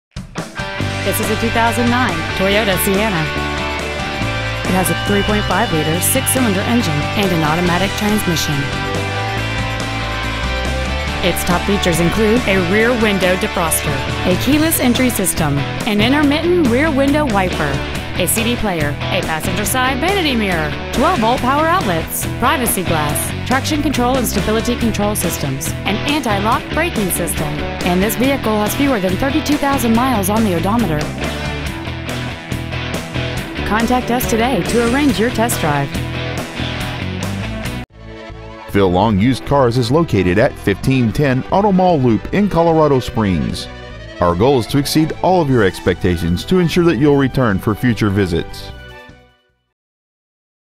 2009 Toyota Sienna Denver CO
Year: 2009
 Make: Toyota
 Model: Sienna
 Engine: 3.5L DOHC 24-valve VVT-i V6 engine
 Trans.: Automatic
 Exterior: Blue
 Miles: 31,255
 Interior: Gray

 2009 Toyota Sienna Colorado Springs CO